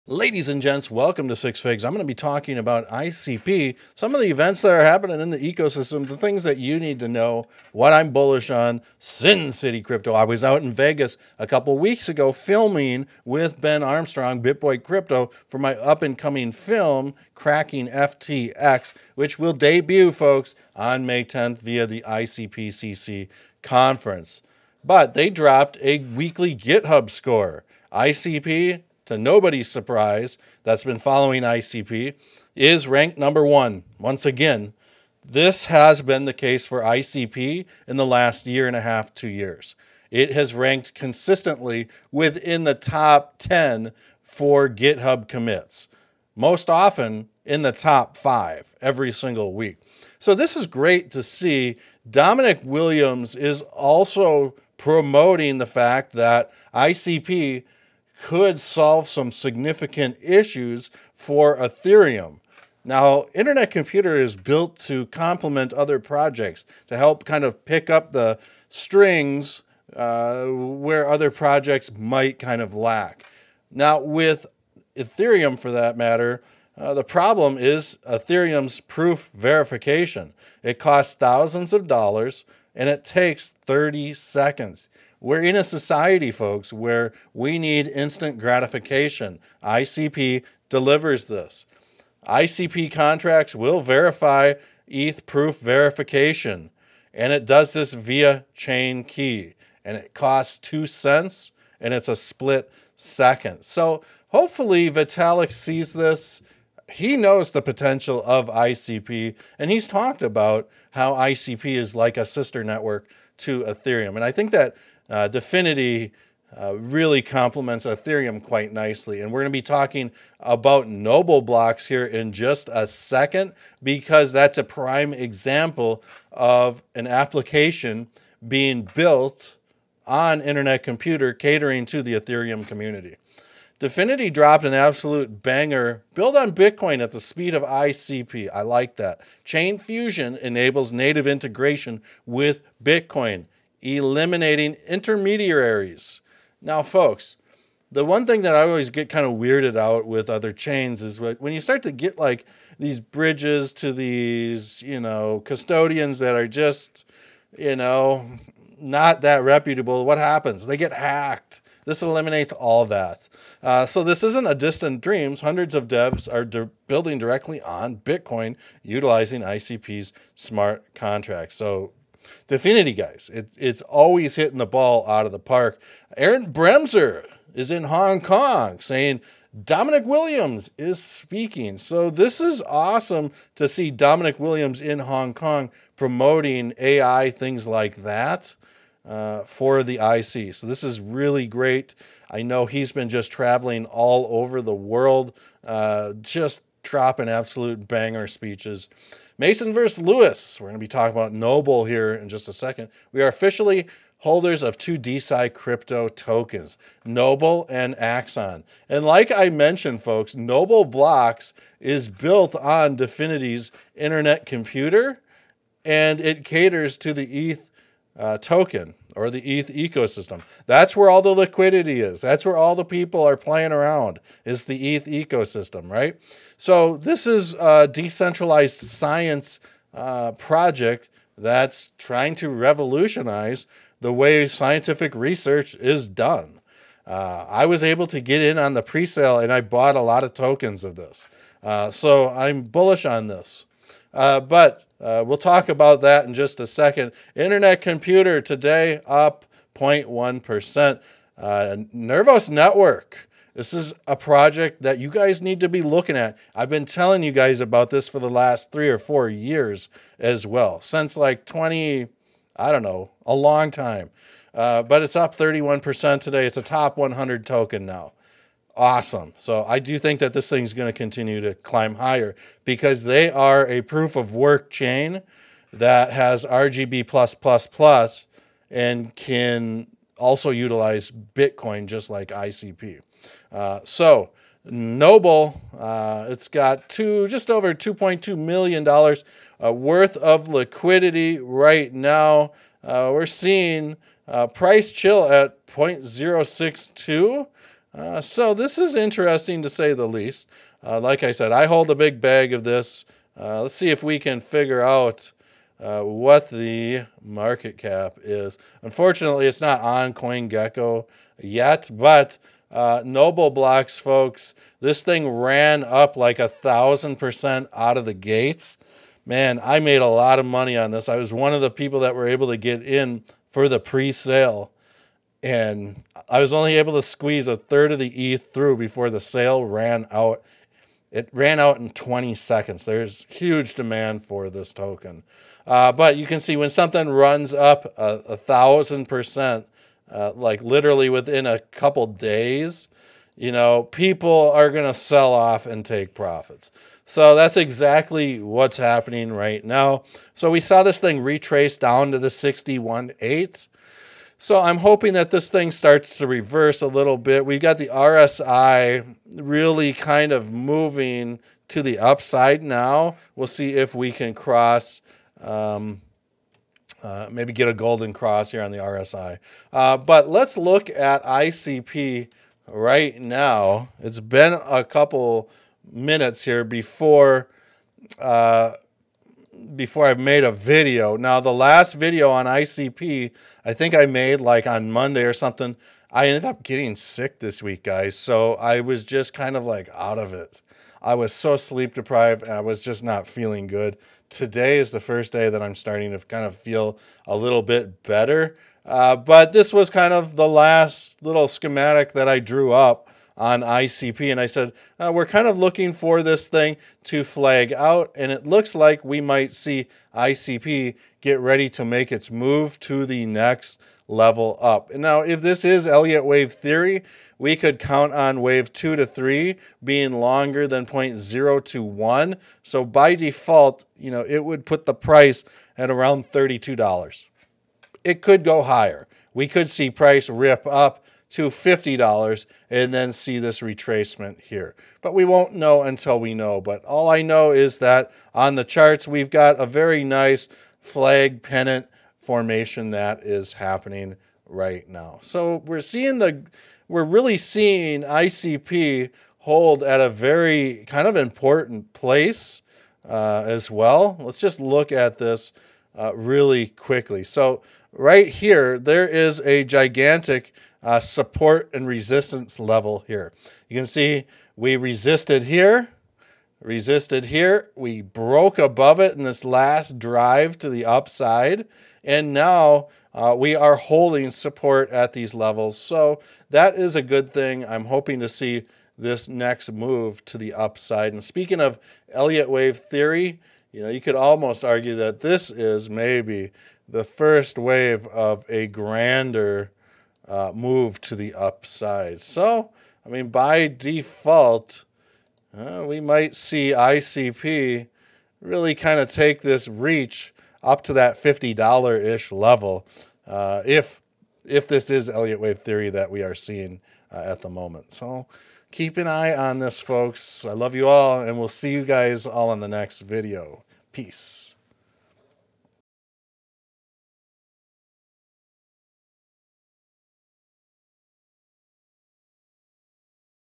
The Internet Computer Will Blow Your Mind | News & Charts
Discover the mind-blowing capabilities of the Internet Computer with the latest news and charts. Learn how this revolutionary technology is changing the way we use the internet!

This is not financial or trading advice, do your own research.
Scammers Are in the Comments, I will never contact you asking for money.